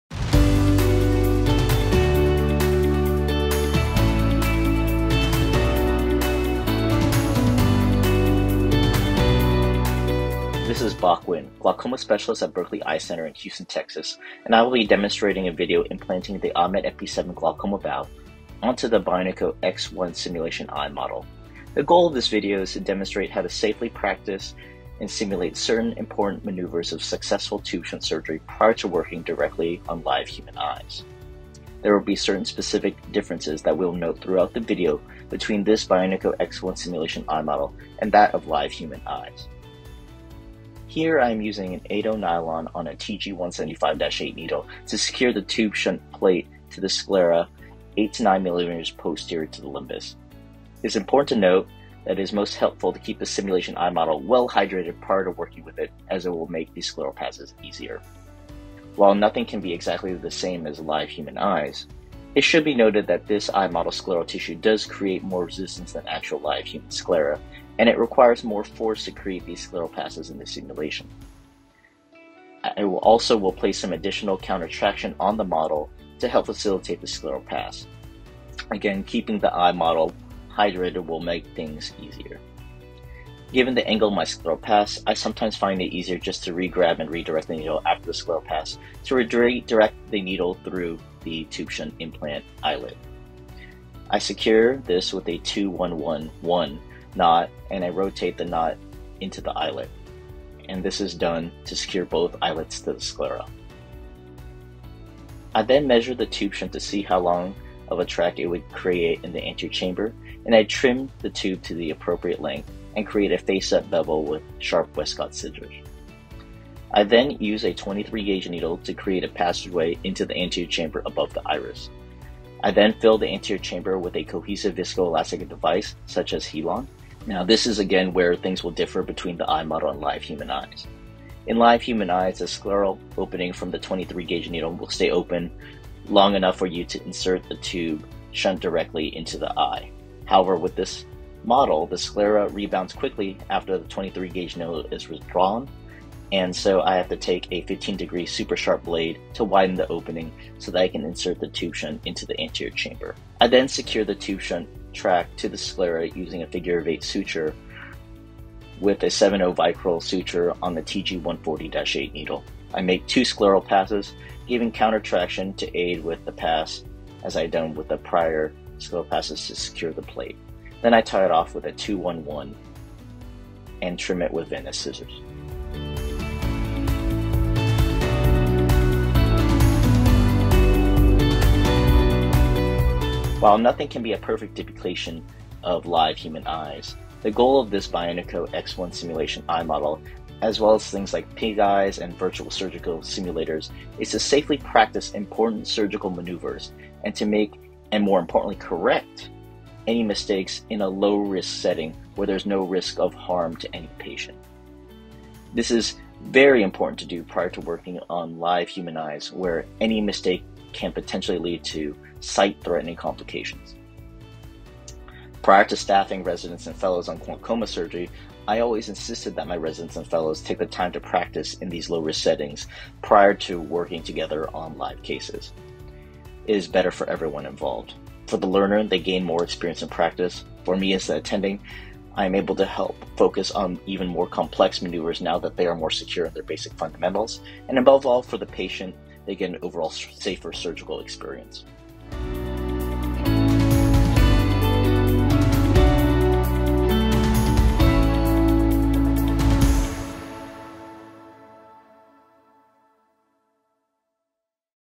BIONIKO EX1 with AHMED GLAUCOMA VALVE (AGV) - by: BAC NGUYEN, MD
A great collaboration video between New World Medical and Bioniko of a tube shunt glaucoma surgery scenario, expertly executed and narrated by Dr. Bac Nguyen.